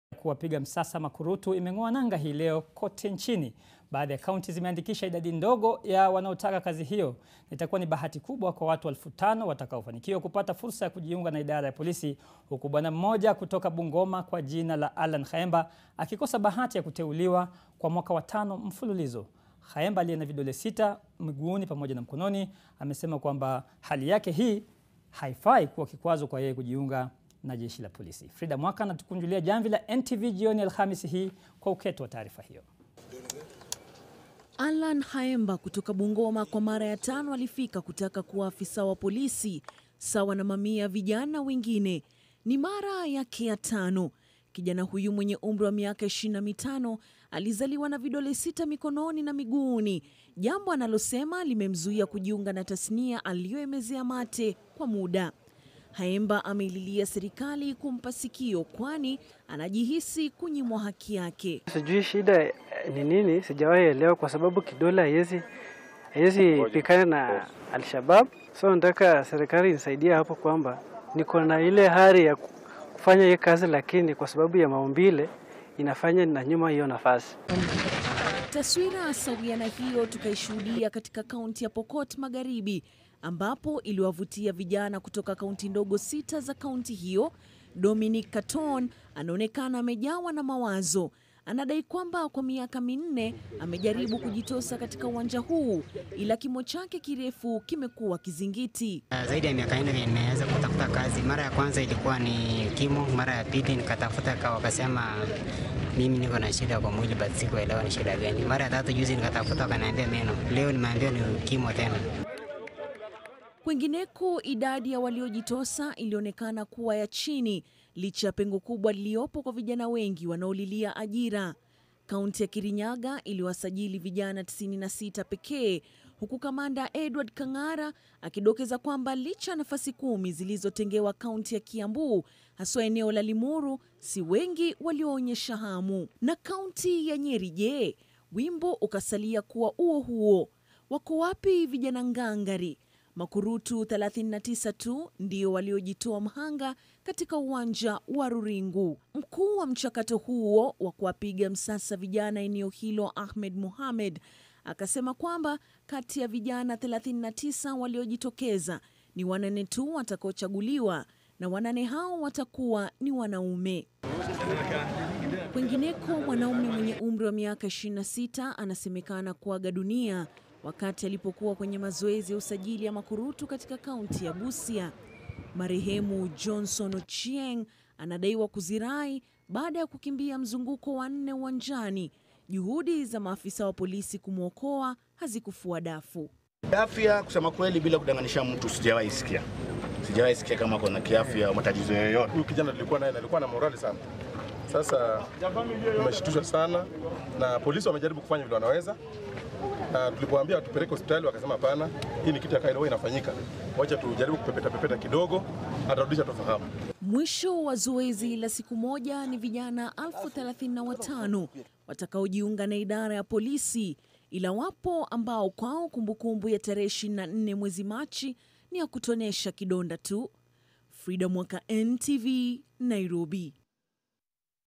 Bungoma: Jamaa akosa bahati ya kuteuliwa kwa mwaka wa tano mfululizo
Shughli ya kuwapiga msasa makurutu imeng'oa nanga hii leo nchini.Baadhi ya kaunti zimeandikisha idadi ndogo ya wanaoitaka kazi hiyo. Itakuwa bahati kwa makurutu 35,000 watakaofanikiwa kupata fursa ya kujiunga na idara ya polisi huku Bwana mmoja bungoma kwa jina Allan Khameba akikosa bahati ya kuteuliwa kwa mwaka wa tano .